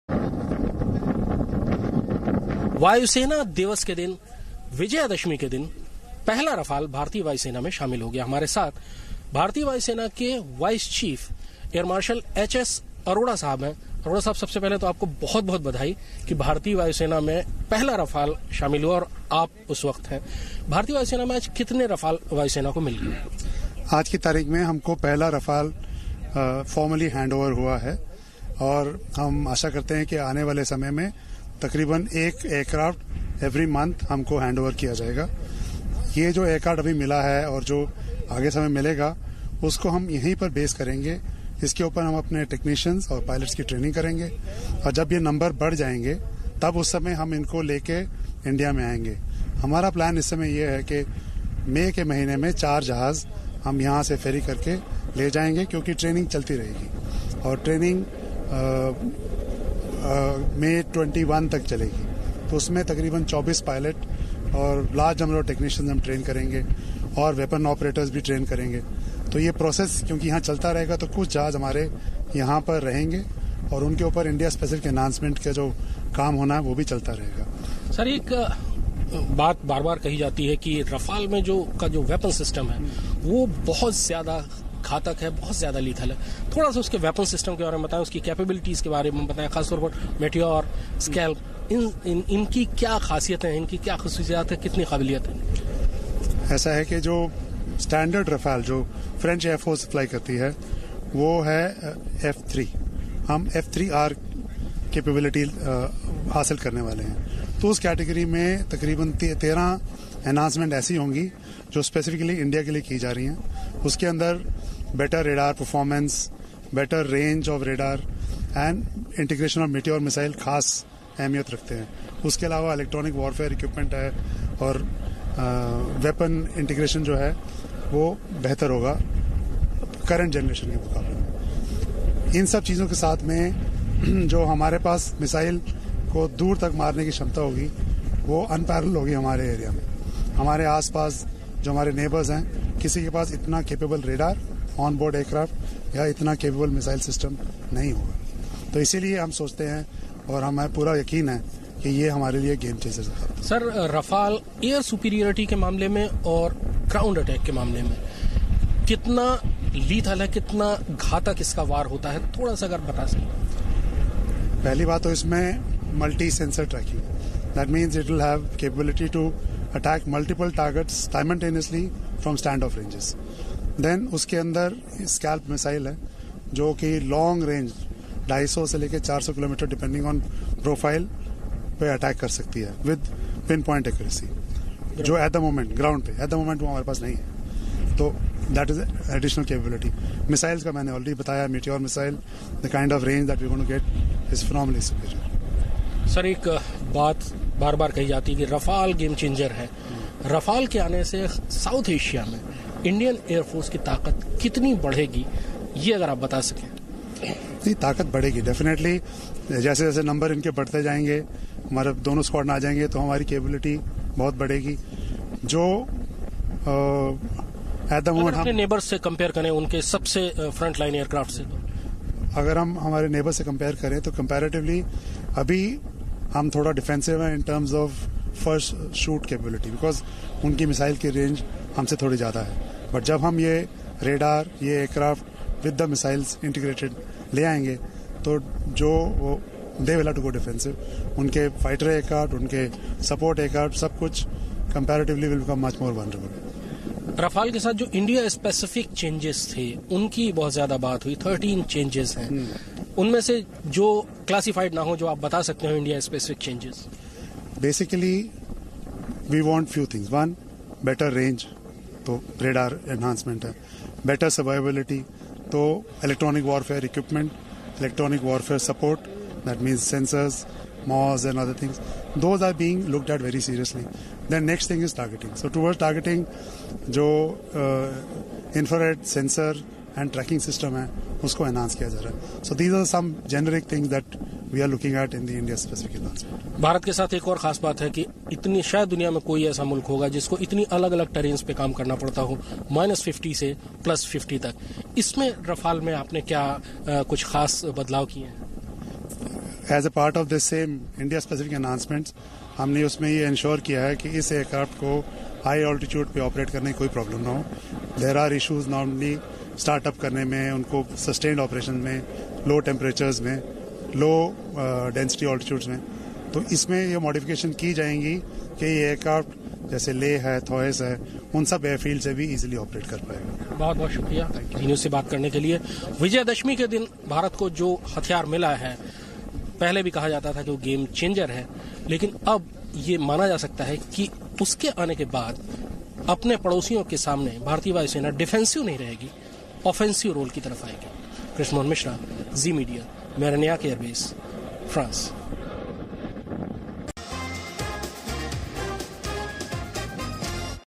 Zee News Exclusive: In conversation with Air Vice Marshal HS Arora
Watch this video for an exclusive conversation with Air Vice Marshal HS Arora.